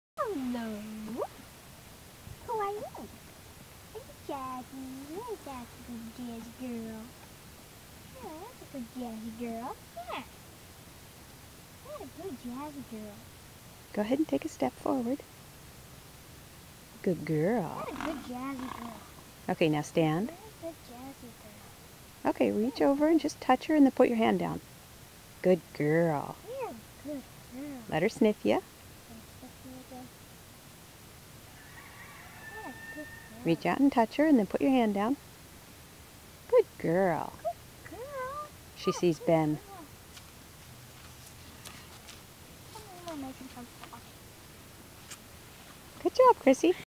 Wednesday afternoon 008.AVI
This is Krissy's first introduction to Jazz...or should I say it the other way around?  I told Krissy just what to do and she did super.  They are going to get along just fine!